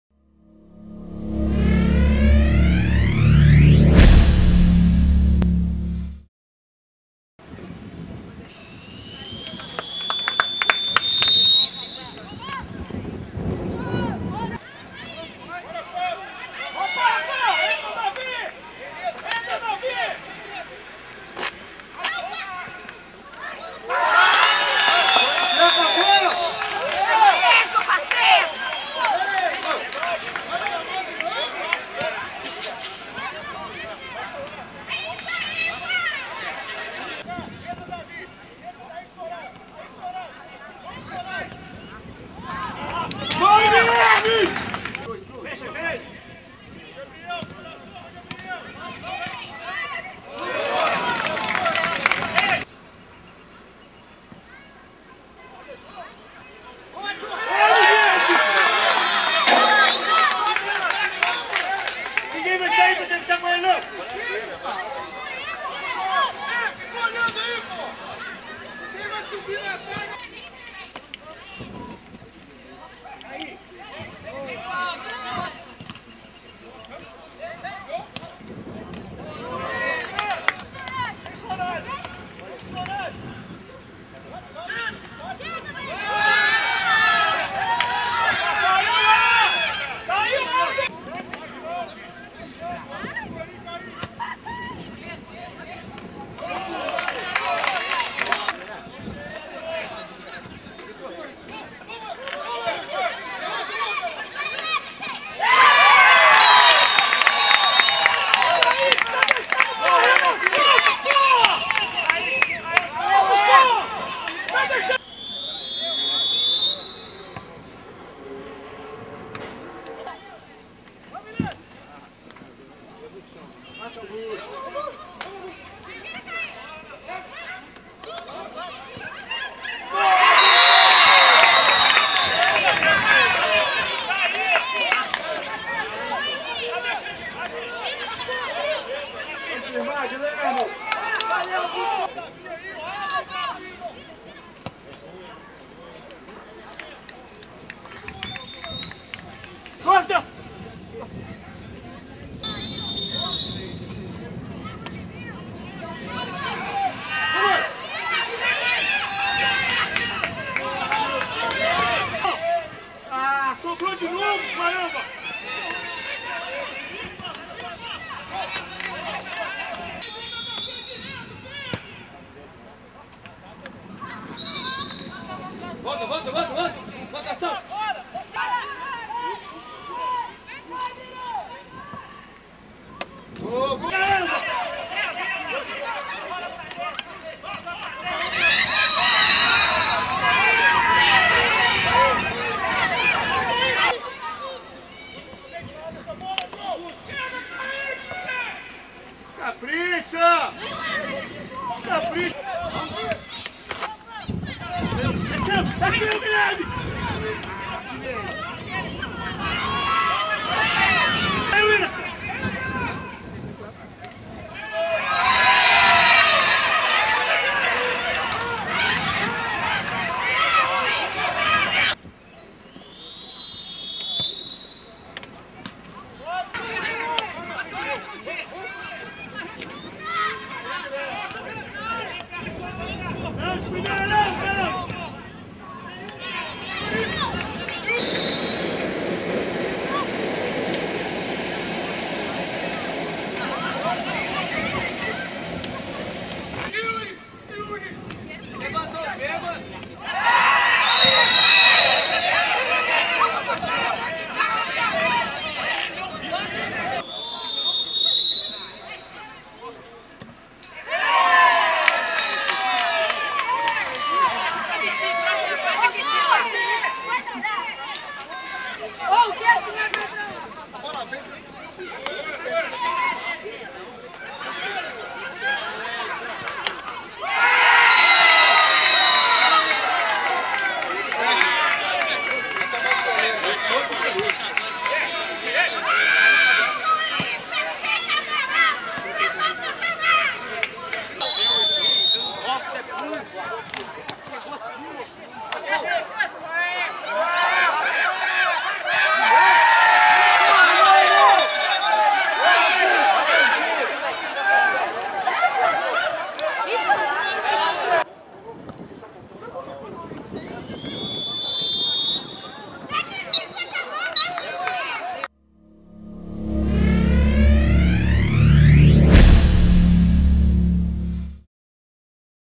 Itabira Esportiva 2 x 4 Santa Cruz/Inter Sub 13 Super Copa IMEF 30-04-2016
Super Copa IMEF de Futebol
Itabira Esportiva 2 x 4 Santa Cruz/Inter - Sub 13
Local: Itabira - MG Campo do Vila Nova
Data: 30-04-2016